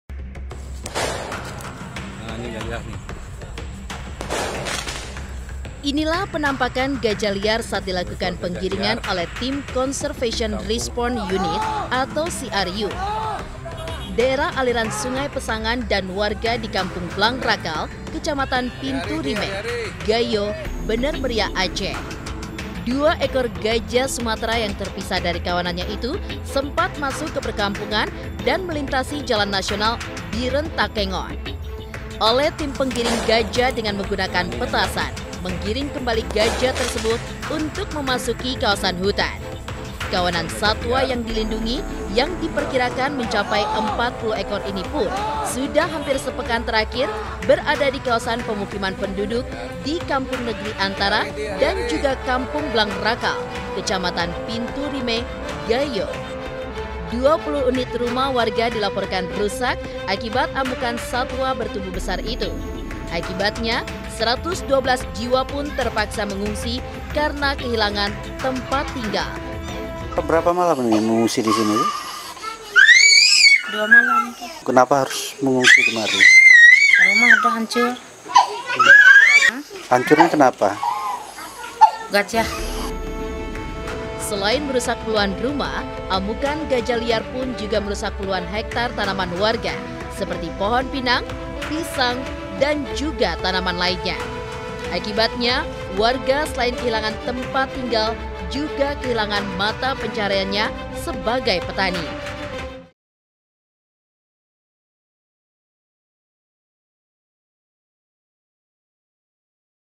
Gajah Liar Rusak Puluhan Rumah, Ratusan Warga Mengungsi
Kawanan gajah liar kembali memasuki kawasan permukiman penduduk dan melintasi Jl Nasional Bireun, Takengon. Dua ekor Gajah Sumatera terpisah dari kawanan dan merusak rumah warga di Kabupaten Bener Meriah, Aceh.

Android: inews.id/android
iPhone: inews.id/ios